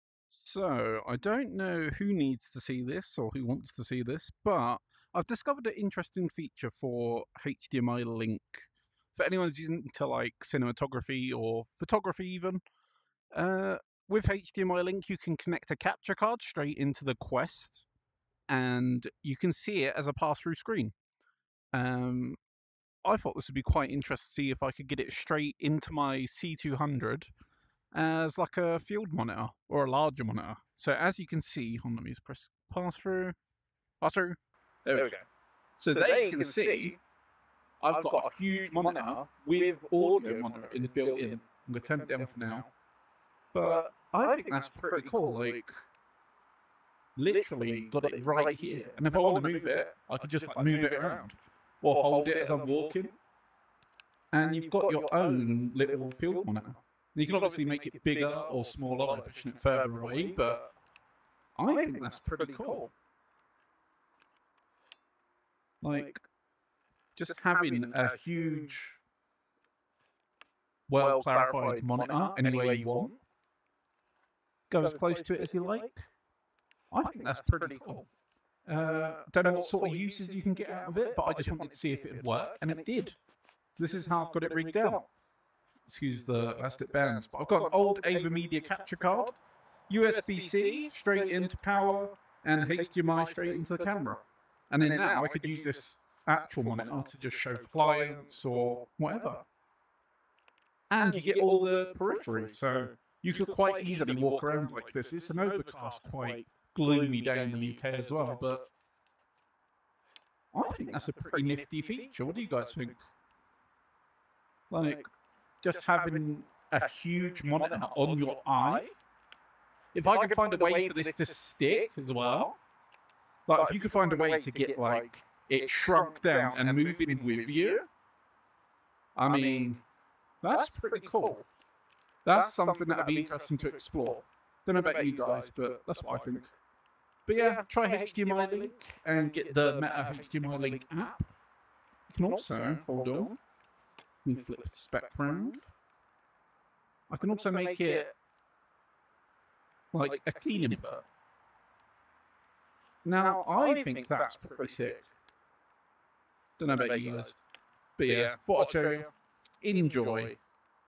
Quest 3: A Filmmakers Dream | Meta Quest HDMI Link + Canon C200
So Meta have recently released the HDMI Link app that allows you to connect a capture card to your headset and play any HDMI sources straight into the headset.

Most people use this for playing console games in the headset, but I decided to see if we could connect our cameras to the headset to turn it into a space sensitive large format monitor.

Surprisingly it worked, but with some caveats.
First one being, you must make sure your camera is recording at the available res of the HDMI input Meta allows (which seemed to max at 1440x900 at 60fps).
This may be fixable, I just don't know how yet, so can only currently record at 1080p 60fps on the C200. Sure there will be workarounds.

Second is that the screen cannot 'stick' on your vision, it's placed in the world like an object so you can move around it. I'd personally like it to be stuck to my face in a specific spot while I move.

Also the recording picked up two audio sources, hence the echo, one was the direct mic input as normal, second was the HDMI passthrough.
Only an issue when recording in the headset ofcourse, nothing on the original camera feed. I should of muted it!

Overall it worked really well, it gives you a customisable direct monitor of your feed (including audio) that you can move and place in the world.
You also have spatial awareness due to the lovely passthrough cameras of the Quest 3.

Anyway, go give it a try, see if you get any use out of it and let me know any tips or tricks you discover!

Gear:
Meta Quest 3
Meta HDMI Link app
Avermedia BU110 Capture Card
Canon C200